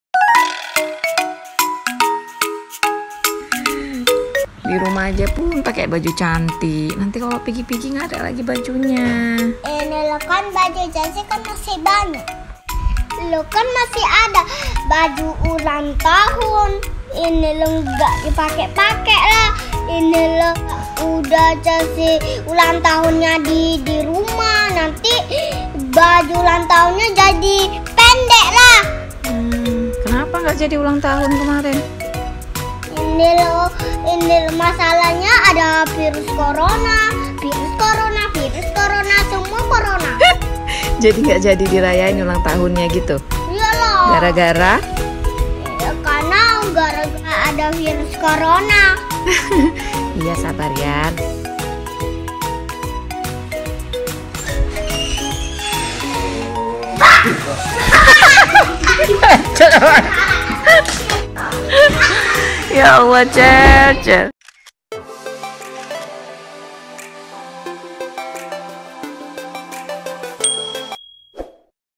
GARA-GARA CORONA
Jadi ceritanya Mami & Chelsea lagi ngobrol
Dan Tiba2 Chelsea ingat ulang tahunnya bulan kemarin yg gagal di rayain di sekolah TK nya gara2 ada pandemi corona .